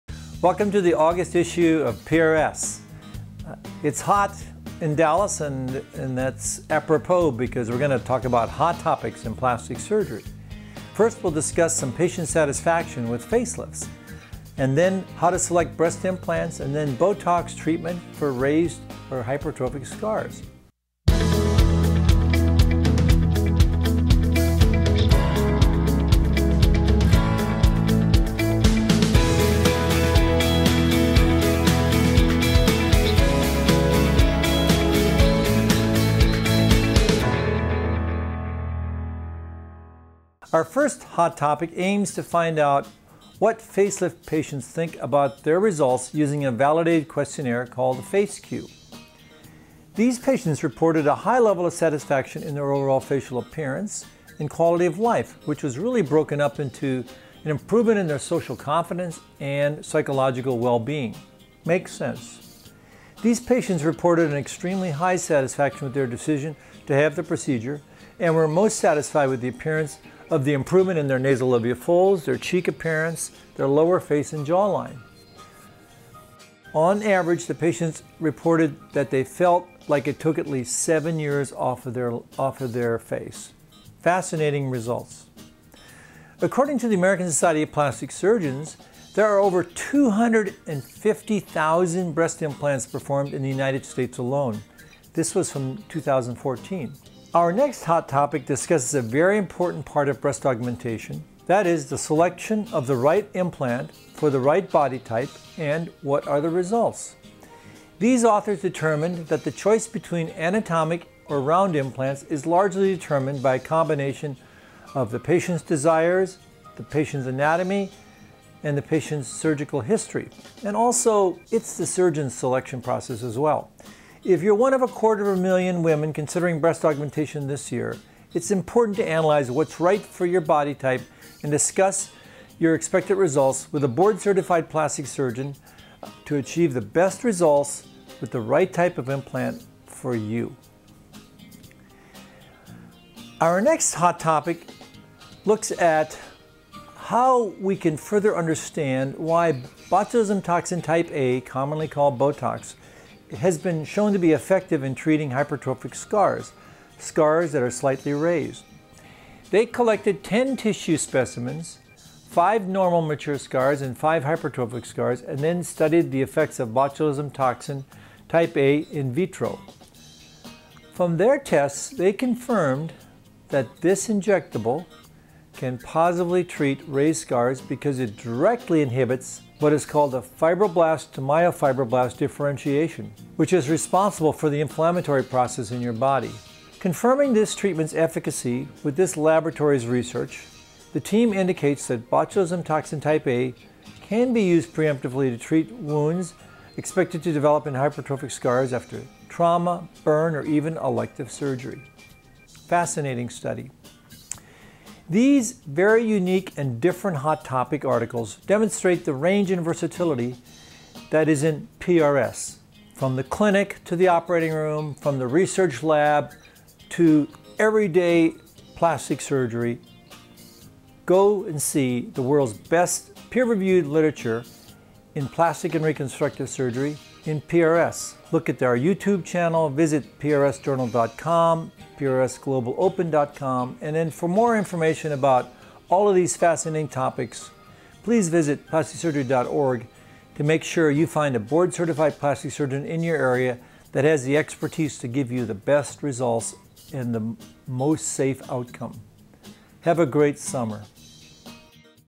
Face Lifts, Implants & Botox for Scars: Plastic Surgery Hot Topics with Rod J. Rohrich, MD
This month Rod J. Rohrich, MD, Editor-in-Chief of "Plastic and Reconstructive Surgery," discusses three very different articles all within the realm of plastic surgery. The first article asks patients what they think of their face-lift. The second introduces a technique for implant selection. The last article tries to explain why Botox is effective in treating hypertrophic scars. Volume 136, Issue 2.

“Anatomical and Round Breast Implants: How to Select and Indications for Use” by Hedén et al

“Effect of Botulinum Toxin Type A on Differentiation of Fibroblasts Derived from Scar Tissue” by Jeong et al: